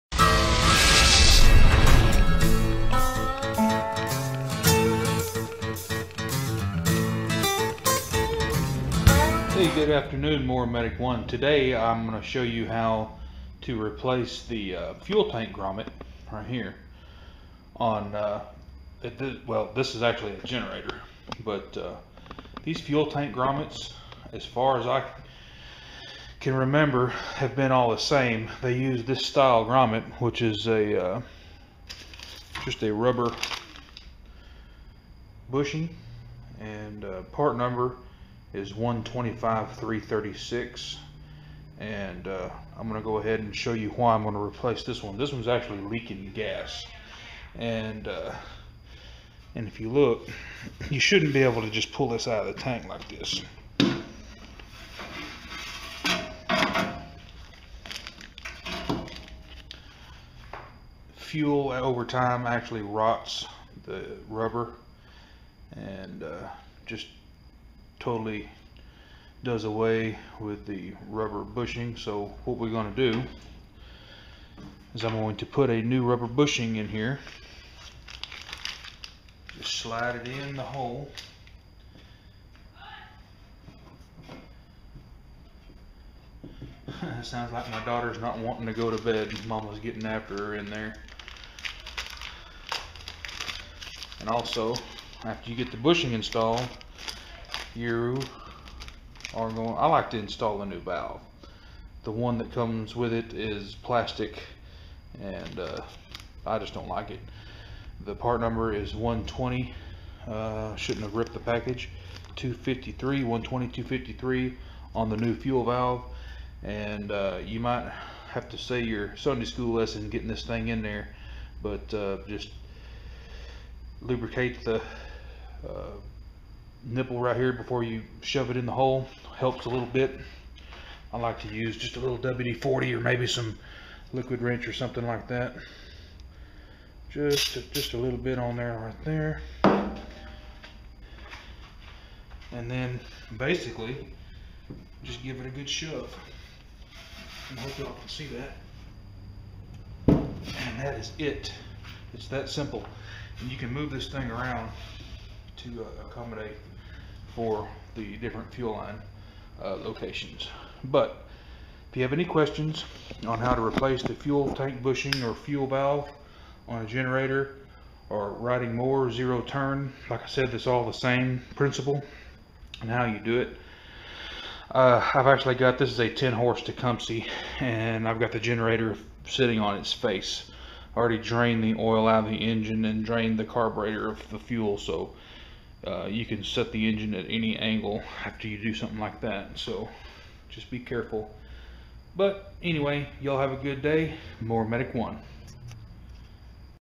HOW TO REPLACE LEAKING FUEL TANK GROMMETS ON MOST SMALL ENGINES AND EQUIPMENT
JEREMY FOUNTAIN
THEMOWERMEDIC1

TREY DRIVER
BIG BUCK MOWERS AND EQUIPMENT
3301 STATE HIGHWAY 121
BONHAM TEXAS 75418

tank valve for sale here!

REPLACE LEAKING FUEL TANK GROMMETS BEFORE ITS TOO LATE...IT COULD POTENTIALLY CAUSE A FIRE AND BLOW UP SO PLEASE BE CAREFUL WHEN MESSING WITH  GASOLINE AND VAPORS